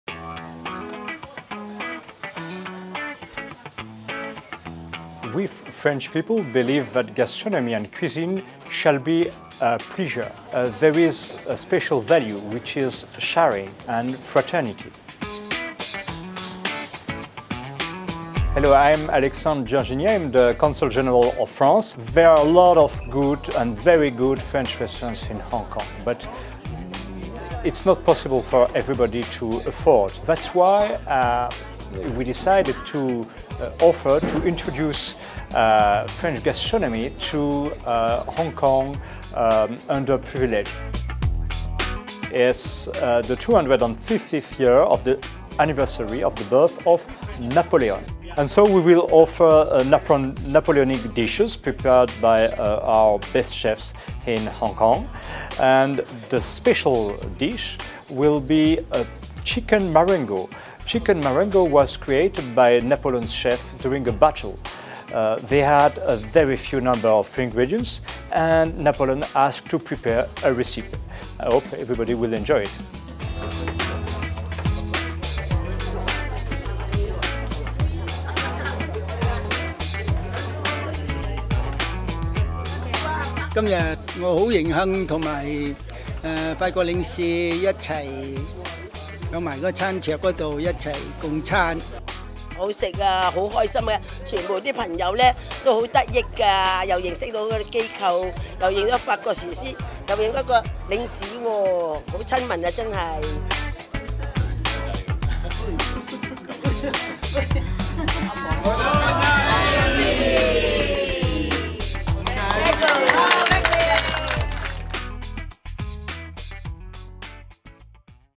French consulate serves cheap meals for Hong Kong’s low-income workers
Hong Kong’s French consulate partnered with the Institut Culinaire Disciples Escoffier (ICDE) to serve a French menu at the Lunch Club on March 20. The Lunch Club, operated by the French Chamber Foundation and Baptist Oi Kwan Social Service, serves meals for just HK$8 (US$1) five days a week for low income workers.